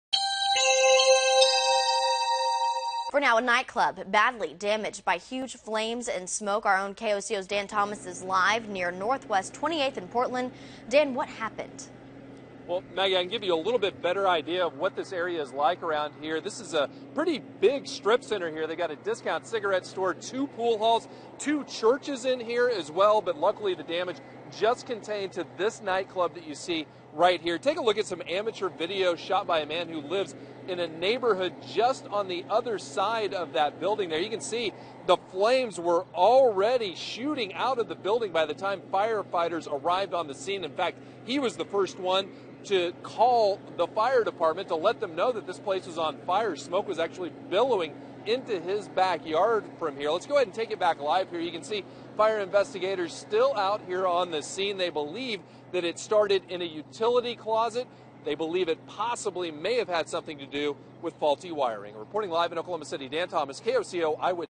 Early morning fire damages business
An early morning fire near Northwest 20th and Portland damages a business.  Dan Thomas reports.